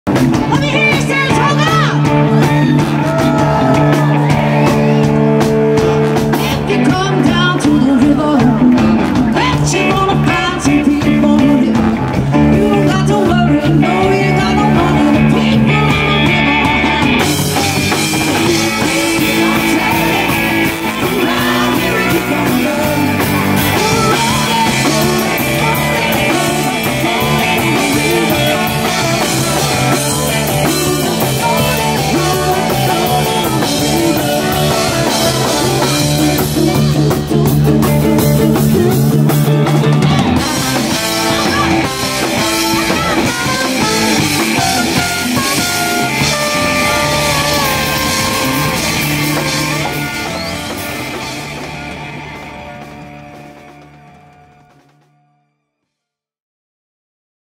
Erin Harkes Big Band
The rock band has expanded to include a full horn section and keys making this the ultimate party band for weddings and galas and whatever else you wanna dance to! Go to erinharkes.com to book the band for 2020! Only a few dates left!